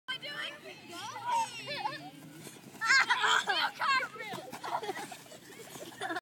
Dog pile fail
We did a dog pile an my friend on the bottom didn't get hurt